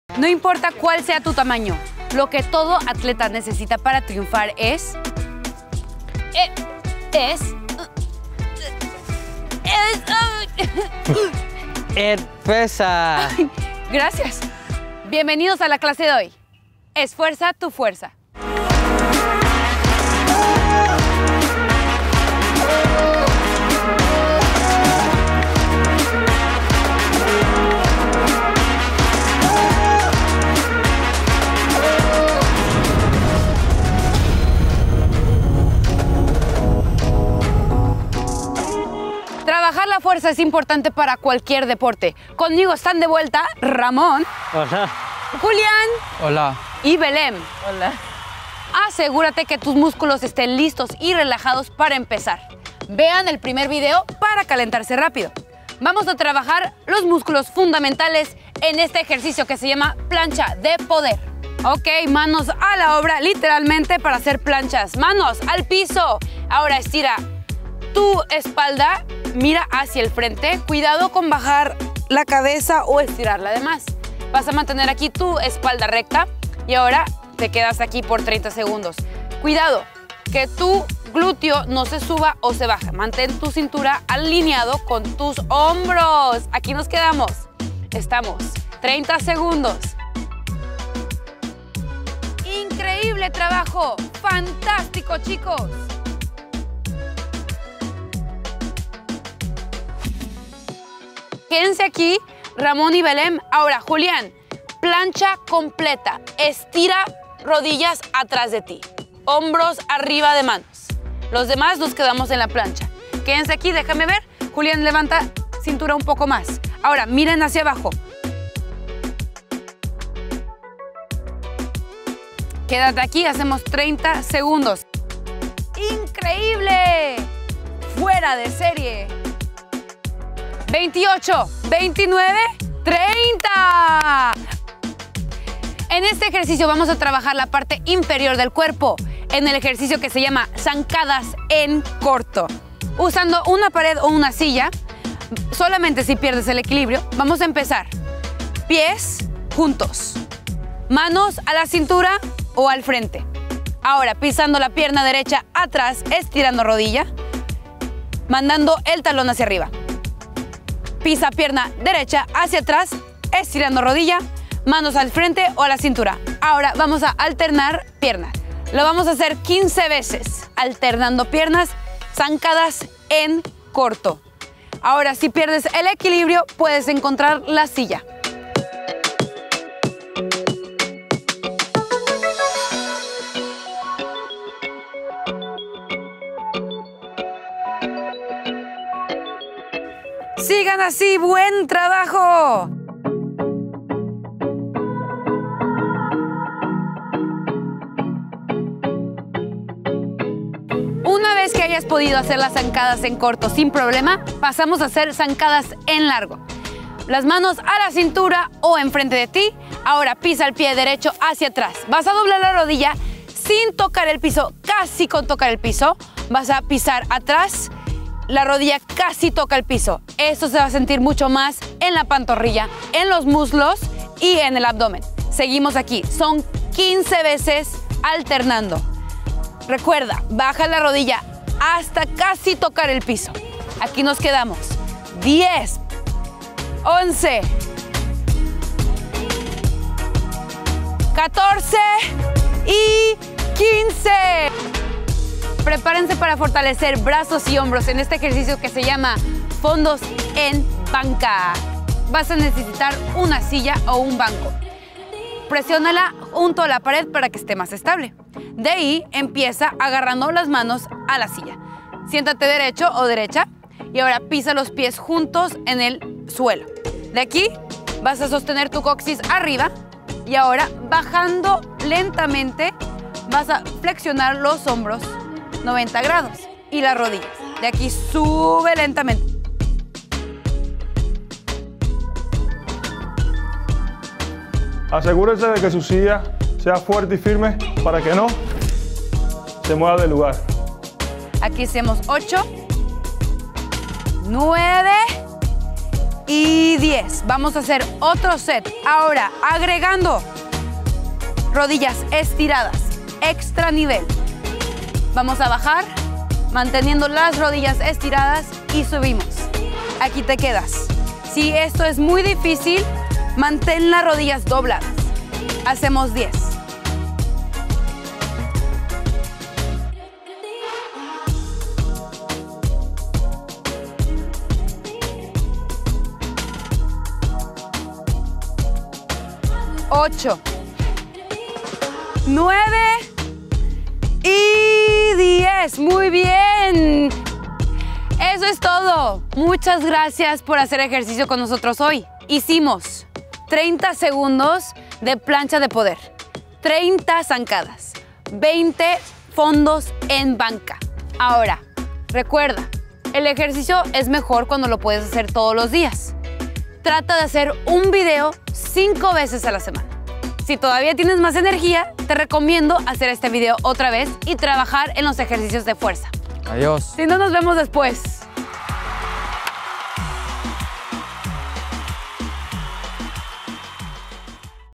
Escuela de Fuerza: Esfuerza tu Fuerza
Prepárate para sentir la fuerza. Hacer ejercicio con este video te va a ayudar a fortalecer los músculos de tu espalda, abdomen, piernas y brazos. Estos ejercicios te ayudan a mantenerte en forma y a desarrollar músculos y huesos sanos.
Este video incluye:
● Plancha de poder
● Zancadas en corto
● Fondos en banca